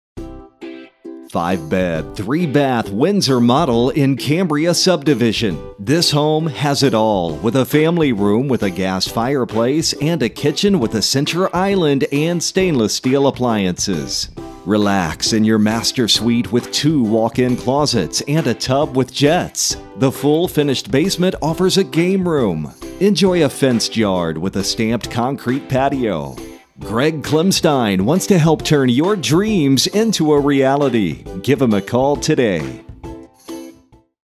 14 Providence Court Cary, IL | ColdwellBankerHomes.com
14 Providence Court, Cary, IL

STUNNING FIVE BEDS, THREE BATHS WINDSOR MODEL OVER 3,000 SQUARE FEET IN CAMBRIA SUBDIVISION ON OVER 0.5-ACRES WITH PRAIRIE RIDGE HIGH SCHOOL ON A PRIVATE CUL-DE-SAC! This Home has it all with Oversized two-Story Living Room, Huge Dining Room, Spacious Family Room with Gas Fireplace, Large Eat-In Kit